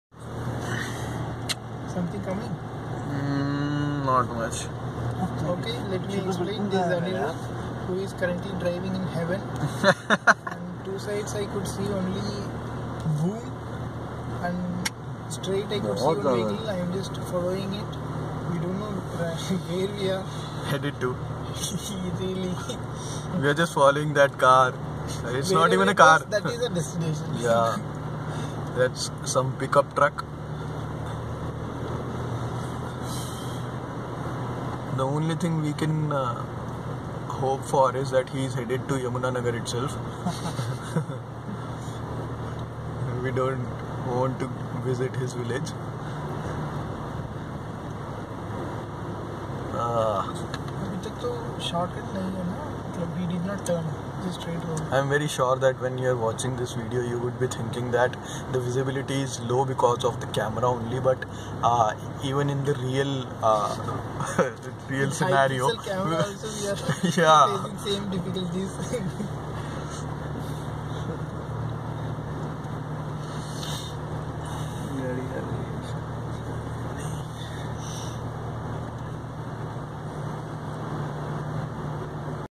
Delhi-Auli-Lansdowne-Delhi trip Day 1 - Early morning drive through fog
Hi friends,

This is a snippet of a video shooted to capture those moments of ecstasy while travelling from Delhi to Auli, at approximately 0400 a.m. somewhere around Indri (Indri-Yamunanagar route).

Do comment about how do you like this video and how do you think I can improve be because this is one of my first time video series.